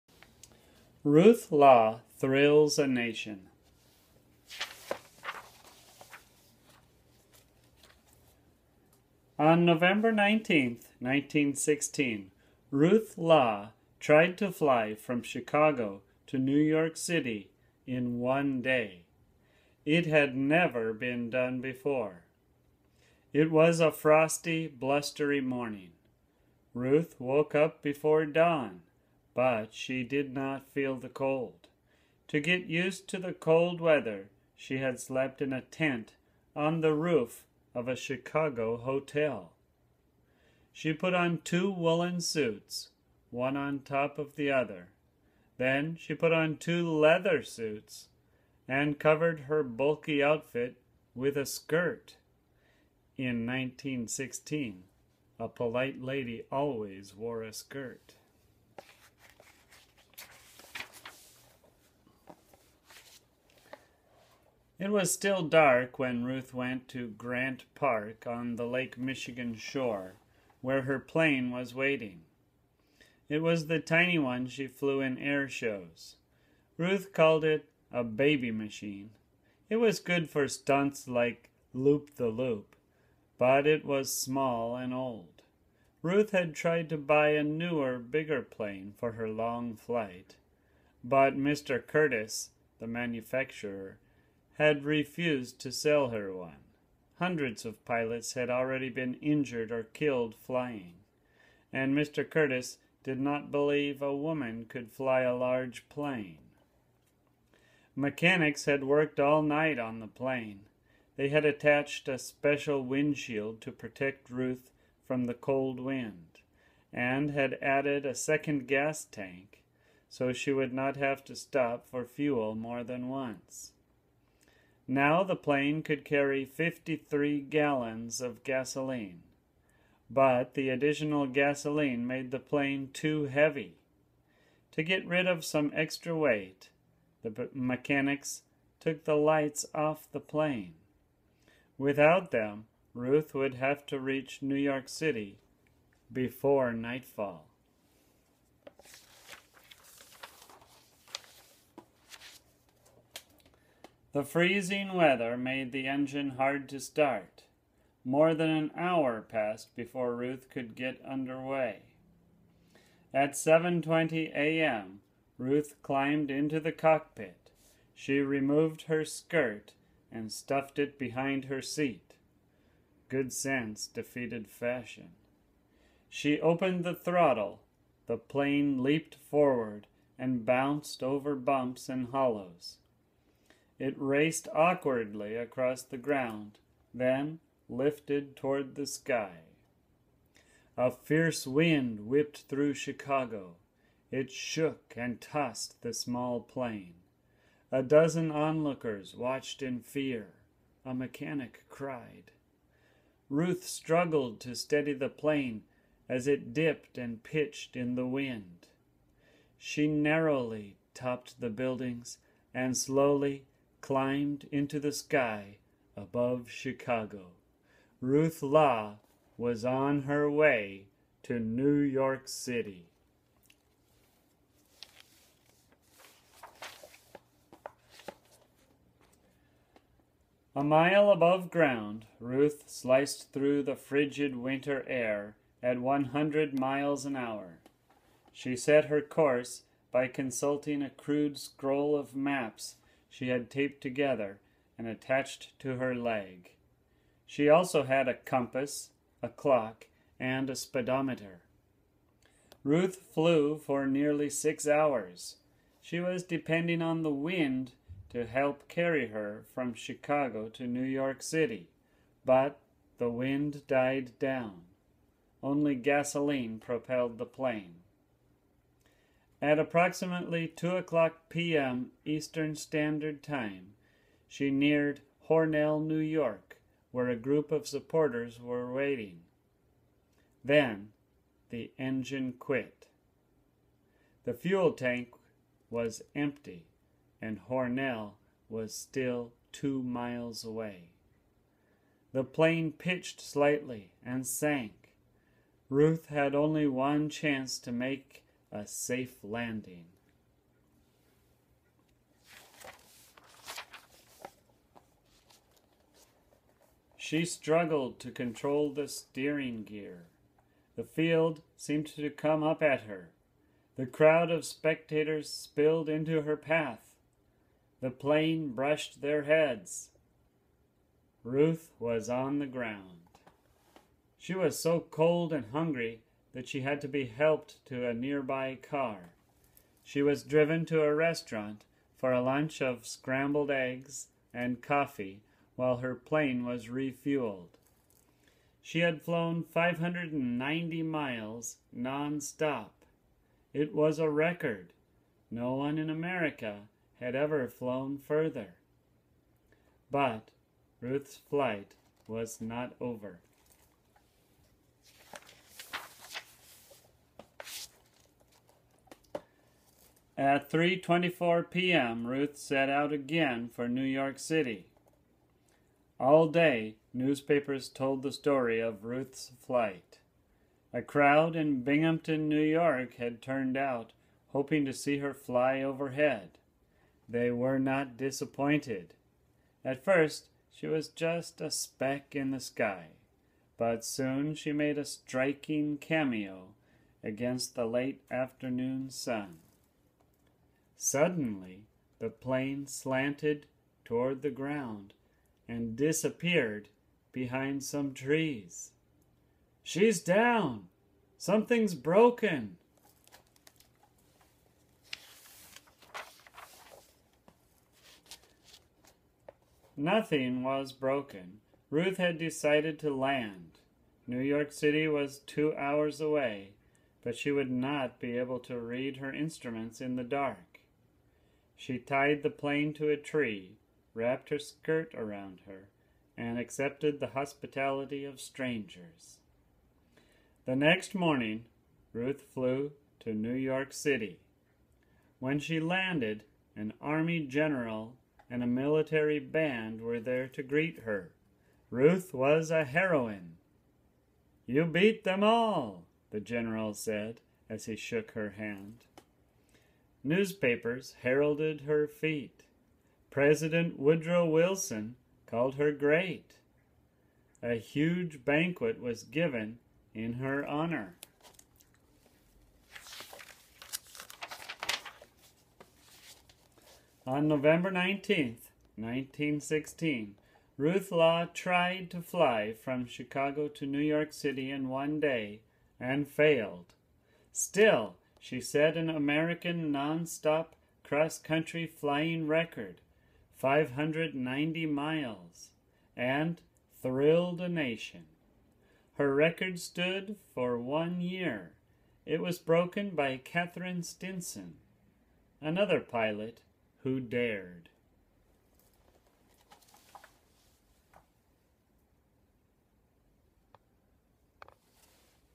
Ruth Law Thrills a Nation - 2.2-15 - Reading
ESL teacher Sky Campbell reads a book for English language learners or anyone who enjoys a good story.